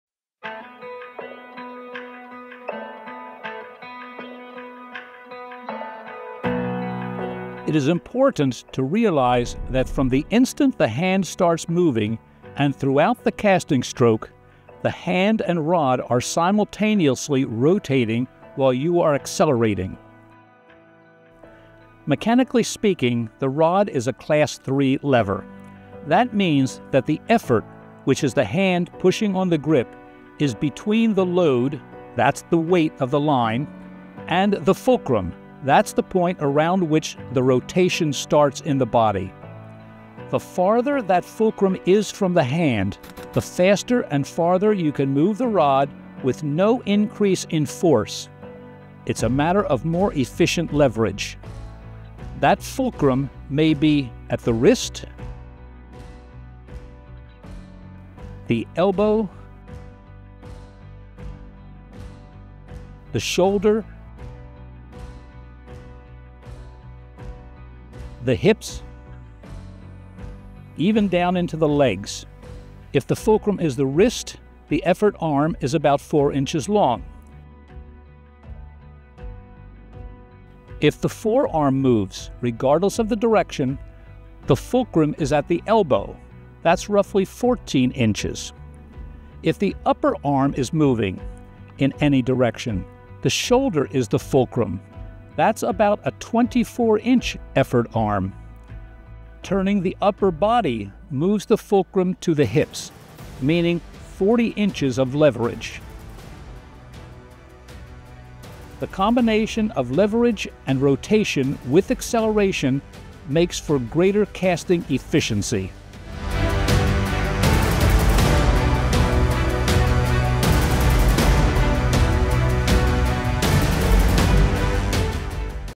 Ed Jaworowski's Functional Fly Casting Part 2: Rotation & Leverage
In the previous video, I discussed the nature of acceleration. Now I'll build on that by looking at an essential feature of acceleration which, for convenience, I'll simply term "rotation," although we could call it "pivoting," or simply "turning."

By using your whole body and rotating at the waist, you use your hips as a fulcrum for better performance. The idea isn’t always to cast longer or harder, the goal is to cast smarter. Golfers, tennis players, and baseball players all use their bodies to increase efficiency while using class 3 levers. Good fly casters should do the same.

Since casting involves kinesiology–the mechanics of body movements–rotation is really an intrinsic part of acceleration, and occurs simultaneously. It is not a separate or additional move that turns over the hand at the end of the cast. The hand starts turning the instant it begins moving, and just as the hand accelerates, moving ahead faster and faster, it is also turning more and more until that instant stop.

Observe your hand, and the rod shaft, when you grasp a rod grip and very slowly move your hand by bending at the wrist or moving your forearm as you would when casting. You’ll note immediately that they are turning from the first instant. The hand and rod rotate in all casts, and throughout every cast during the time it takes to make the stroke. Just as you can’t load the rod without accelerating, you can’t load it without rotation.

In the next video, I’ll explain another factor I call “the critical angle,” which makes casting still easier and more efficient.